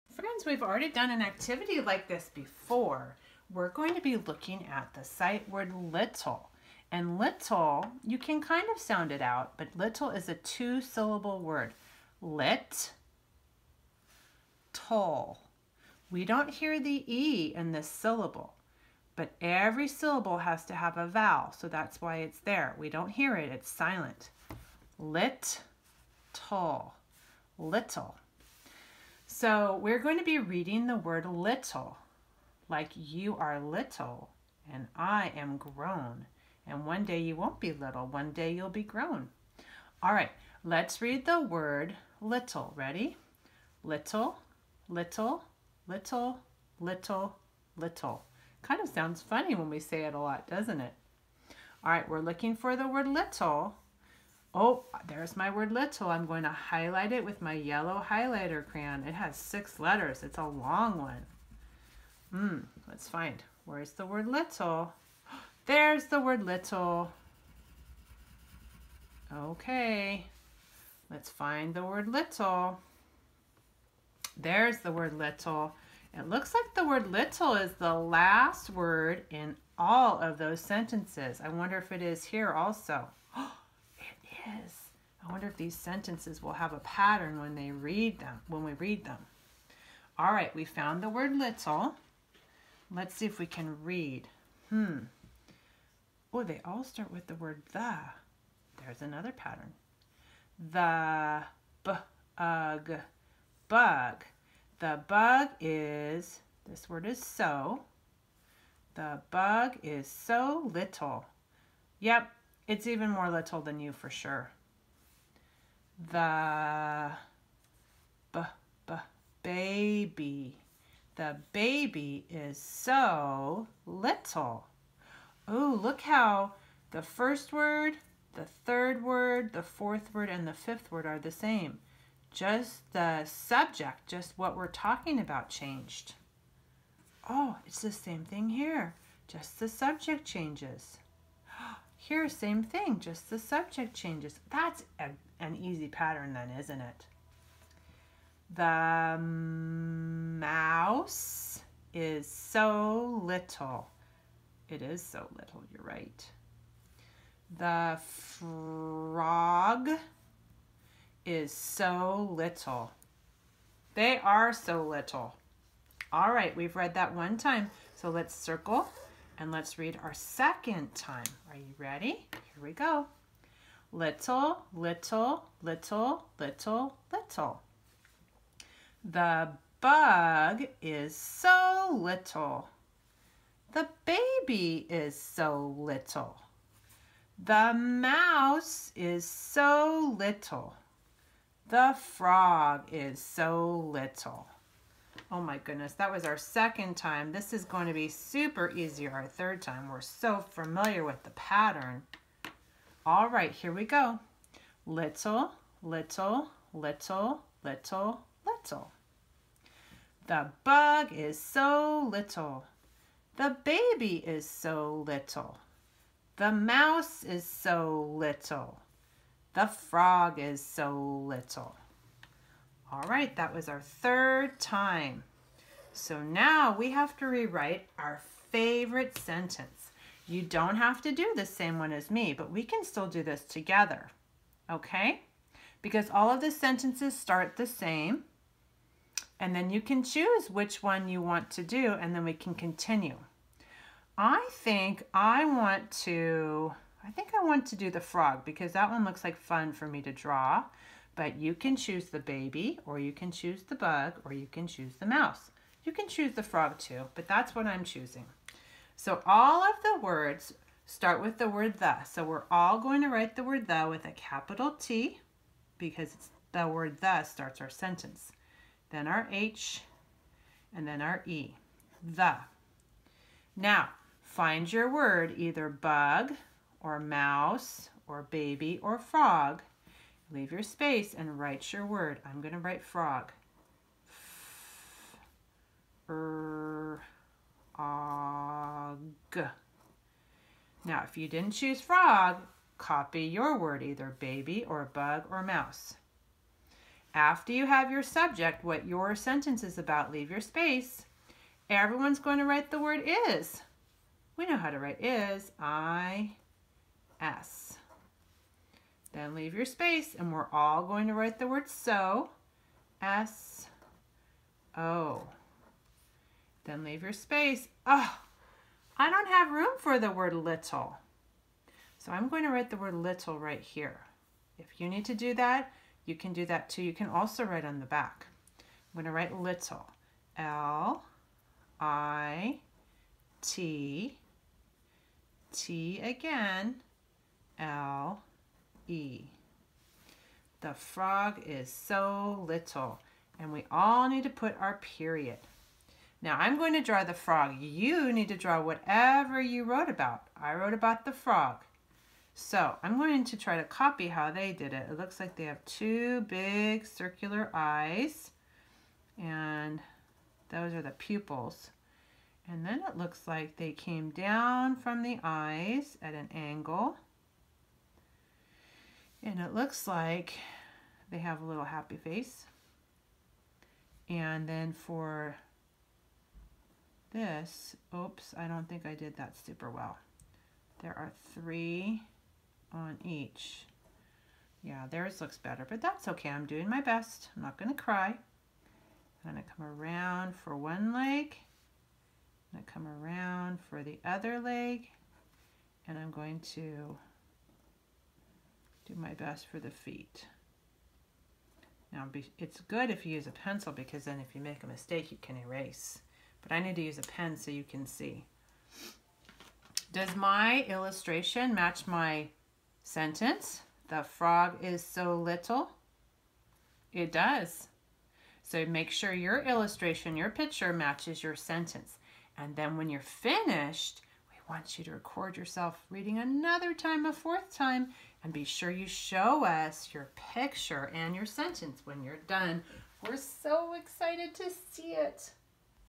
I Can Read! "little"
Read and write sight word "little"